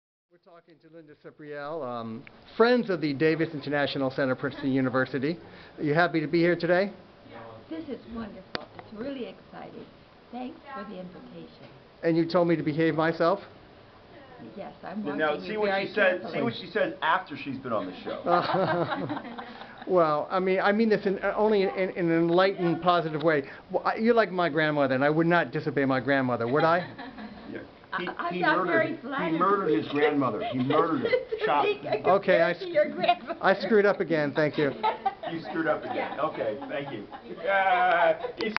Linda Sipprelle, President of Friends of Davis International Center; English tutoring program
The runner, diplomat, community icon,, must use all her emotional skill to deal with Adam's next foot in the mouth comment. Co-host Kurt lends support to Linda during her ordeal